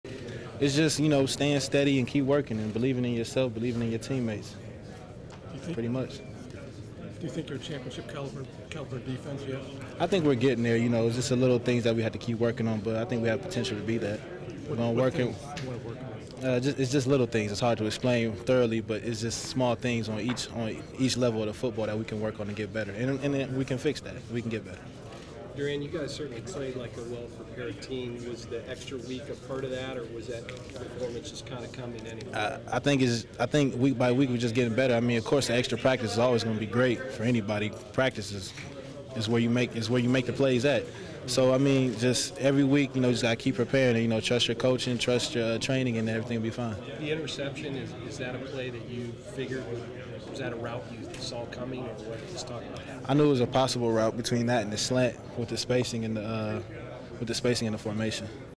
Ohio State Sr. CB Doran Grant 10/18/2014
Buckeyes CB Doran Grant after OSU's 56-17 win over Rutgers